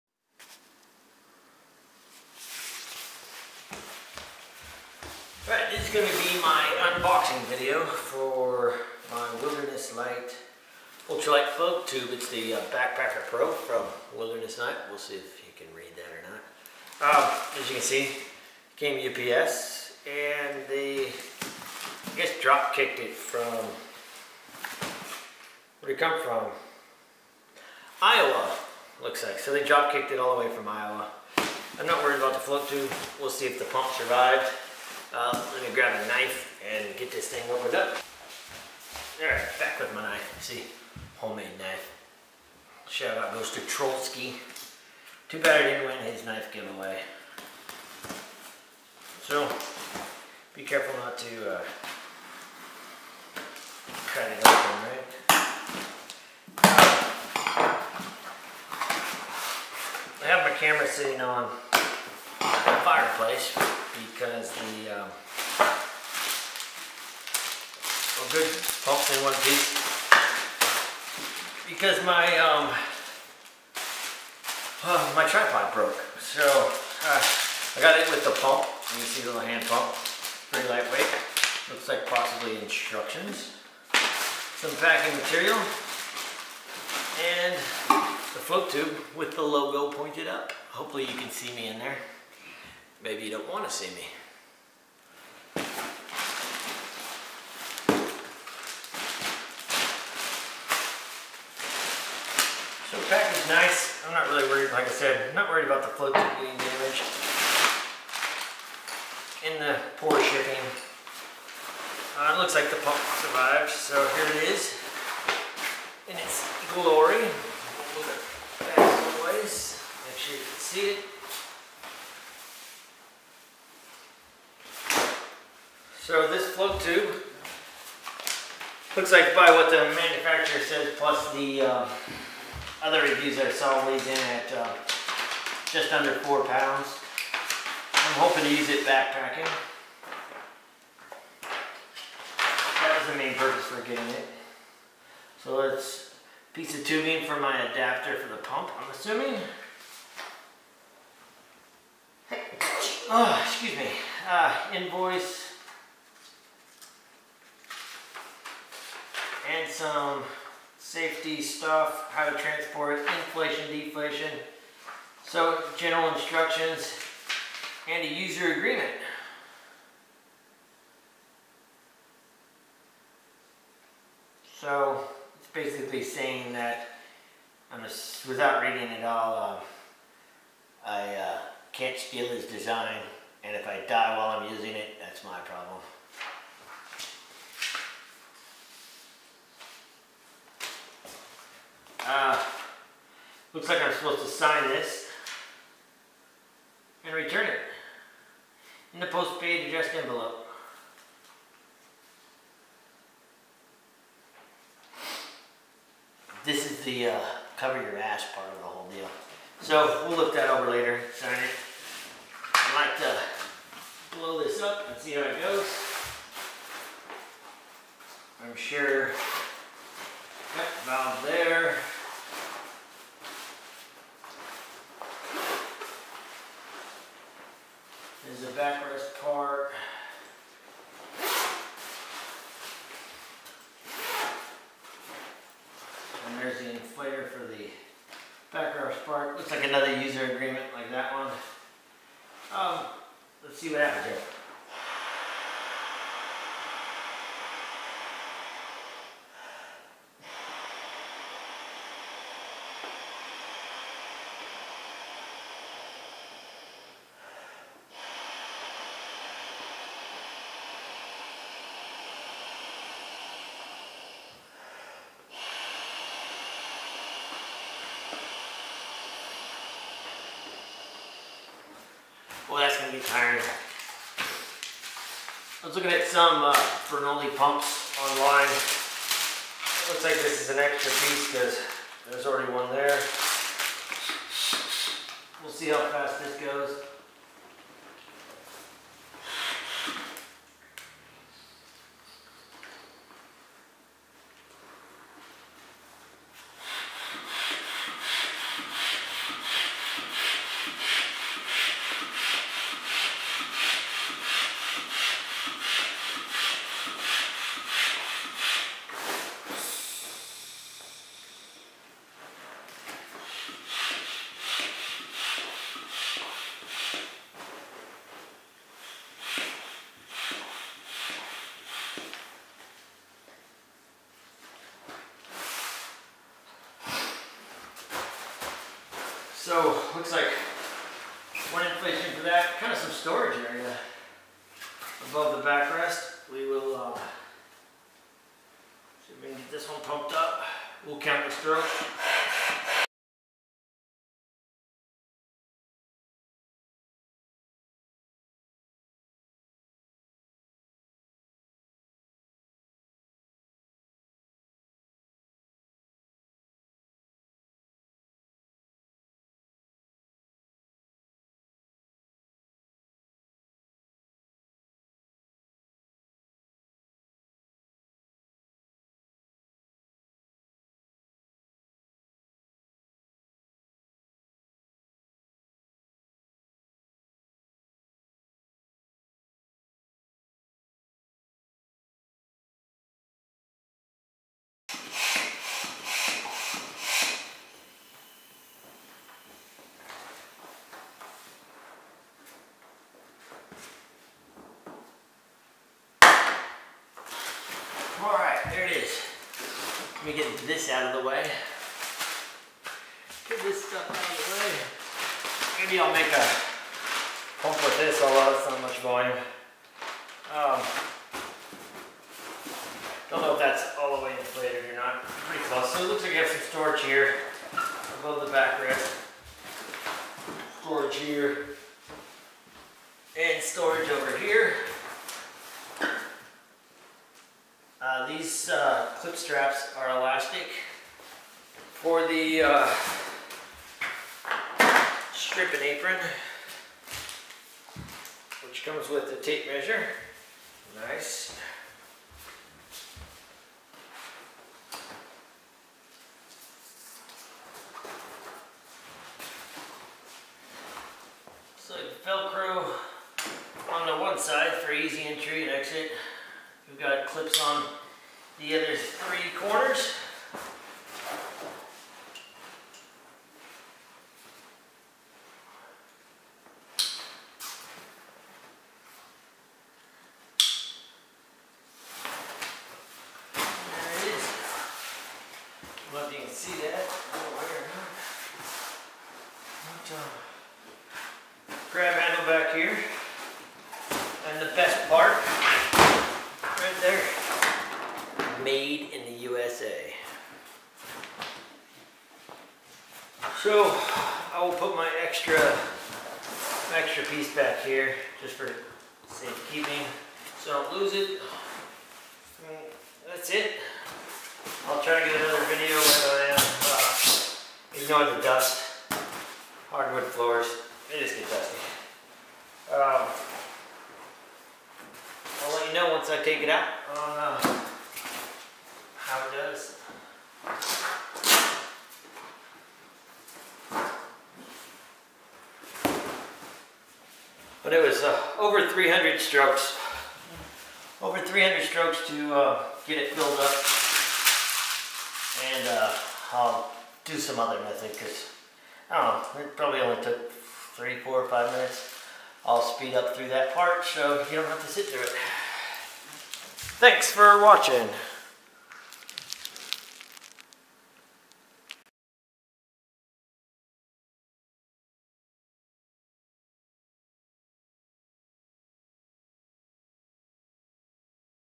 Wilderness Lite Backpacker Pro Unboxing
Unboxing and inflating my new Backpacker Pro Ultralight Float Tube
The thumbnail picture is from the website.